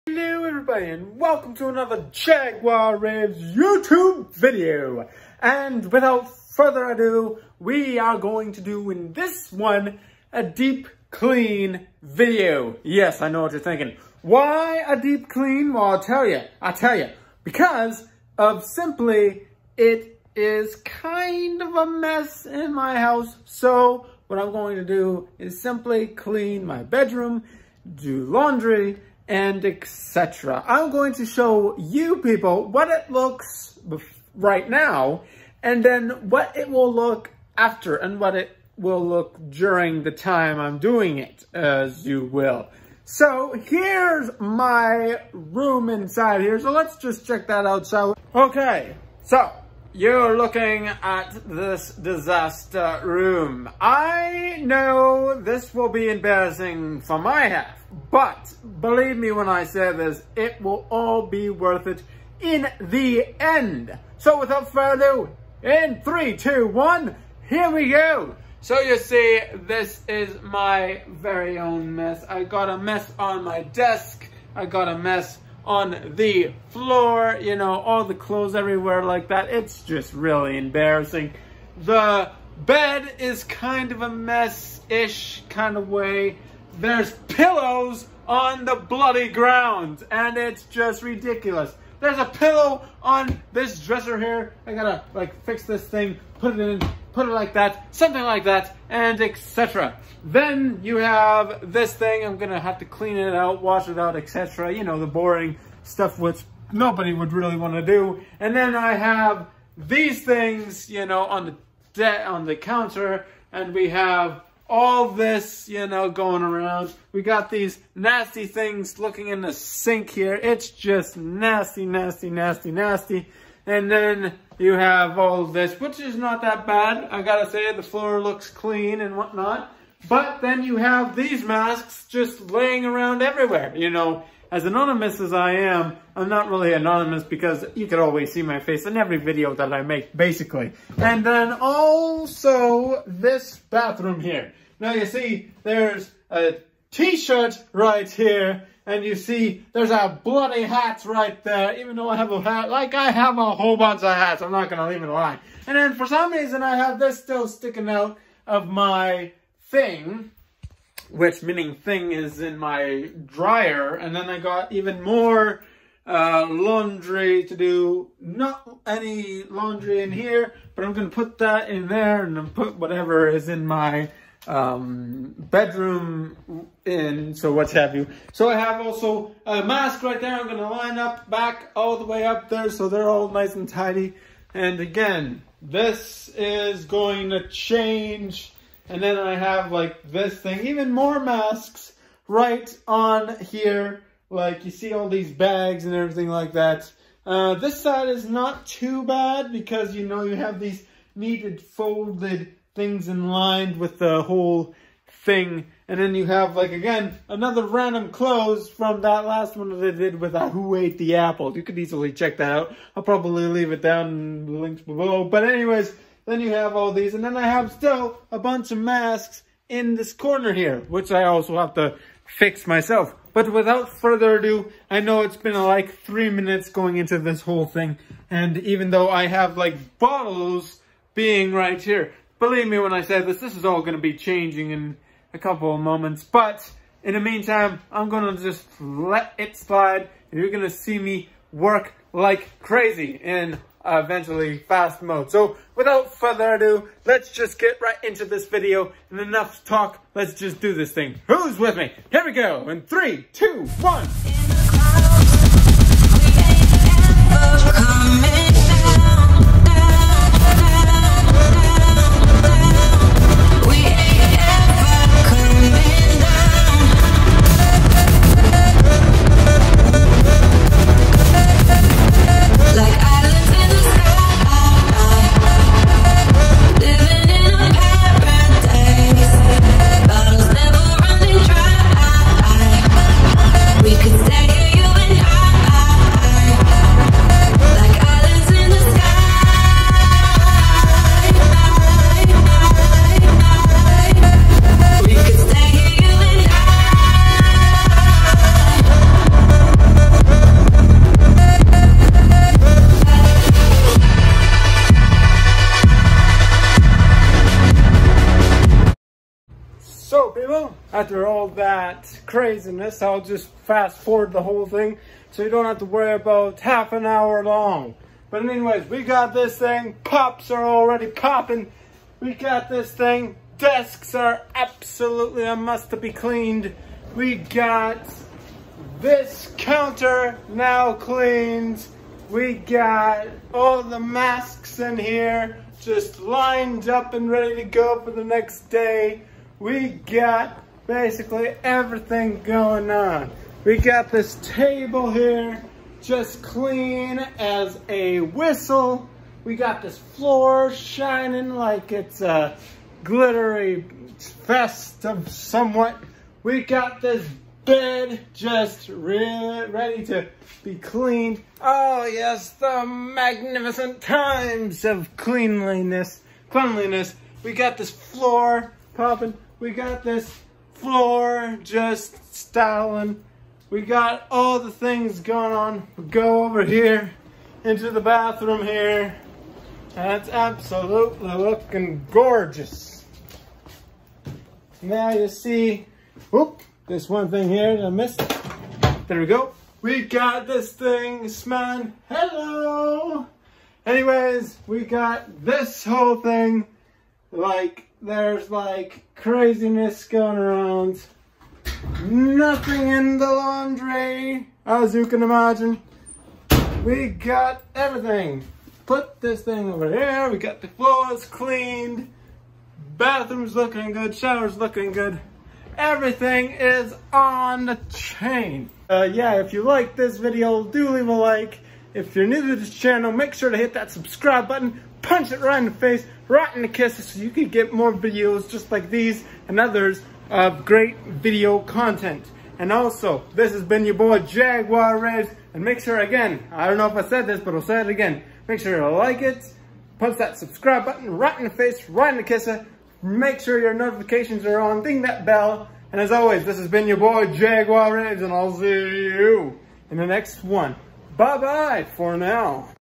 Doing a Deep clean of my house
Watch as I engage on doing a deep clean of my house.